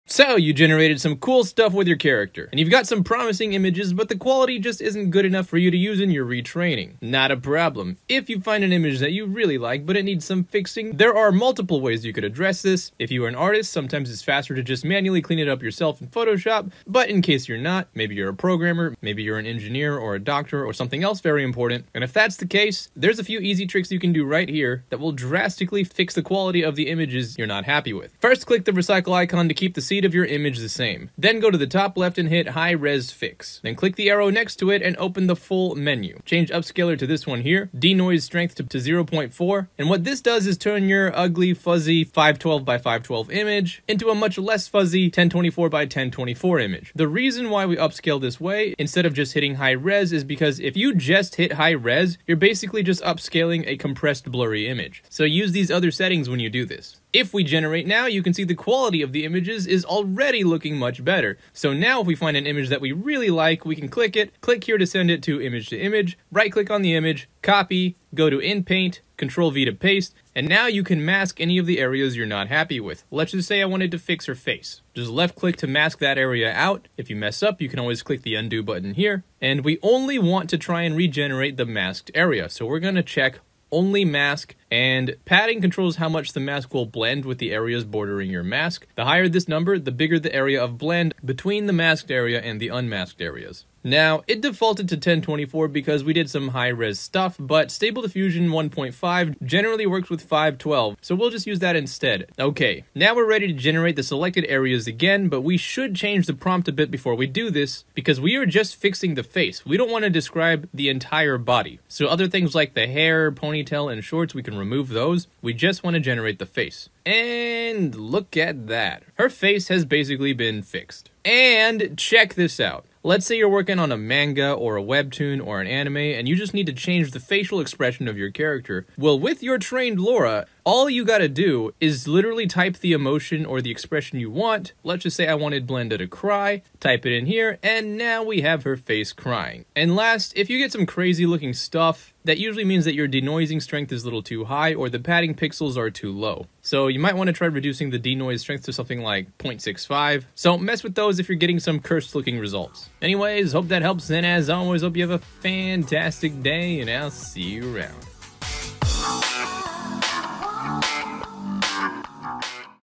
Stable-Diffusion AI: Fixing Training Images (In 3 Minutes!)
Here's a fast/easy way to fix broken images after training -
Big Shout-Outs to Le-Fourbe for walking me through the process!!